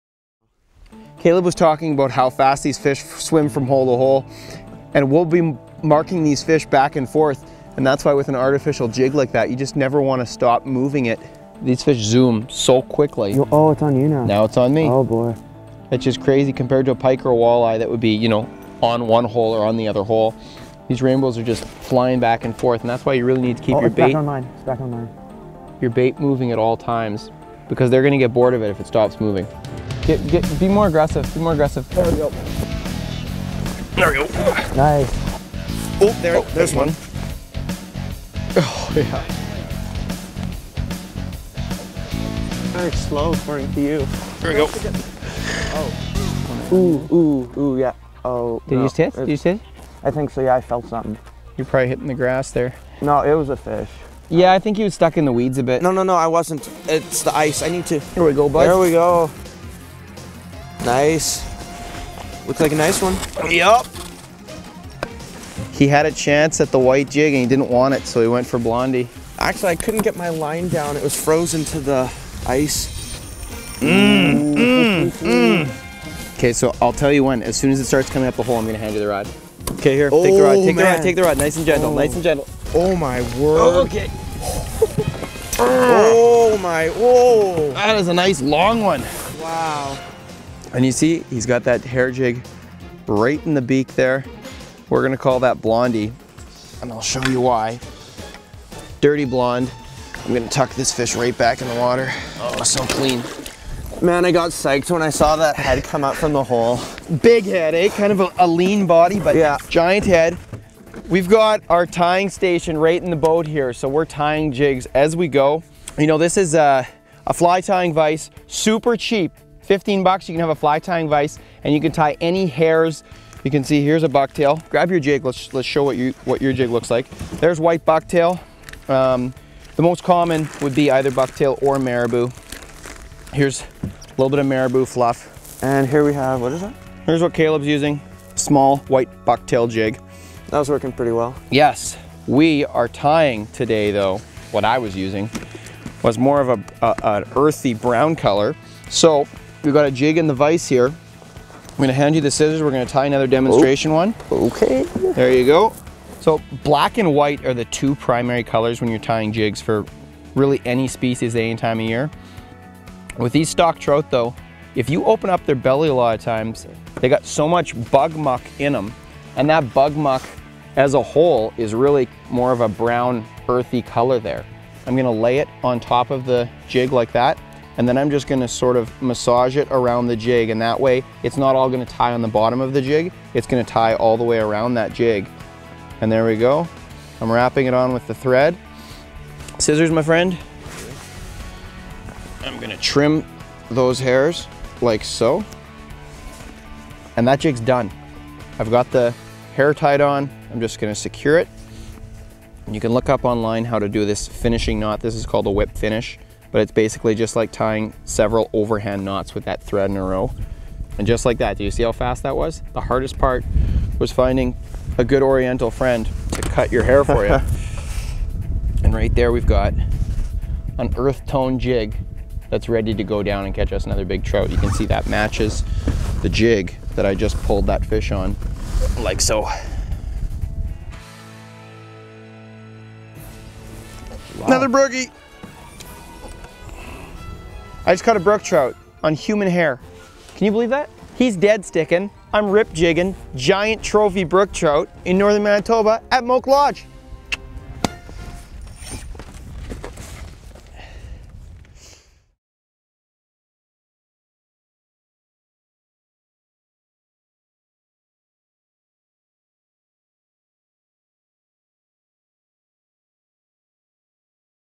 Hair Jigs for Ice Trout - Uncut Angling - March 20, 2014
Aaron and Caleb are on some hot Rainbow Trout action while fishing at Moak Lodge. They demonstrate how to tie your own hair jigs - literally. This is an extremely effective presentation for every fish that swims and making them yourself is easy and enjoyable.

Frabill Ice Hunter "Haataja" Rod Combo #6776
Strikemaster Honda 35cc Lite 4stroke Ice Auger